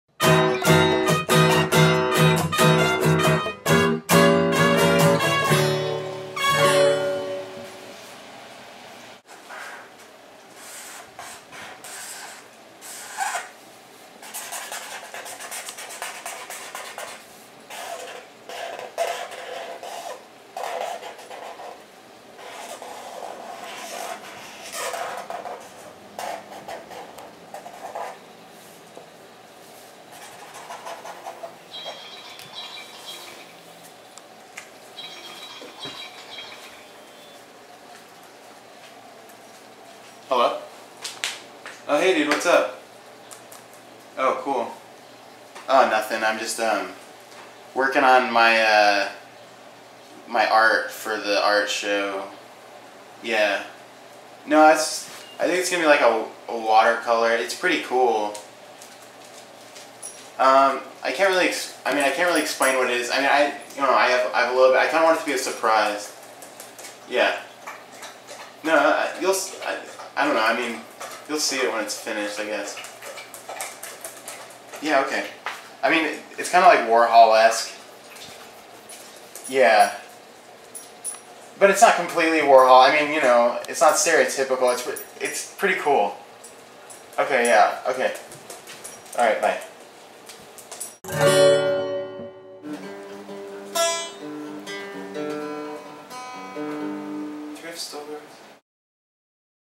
Lowbrow Hipster Episode 6: Art for the Art Show
Shemp is busily preparing for an upcoming art show.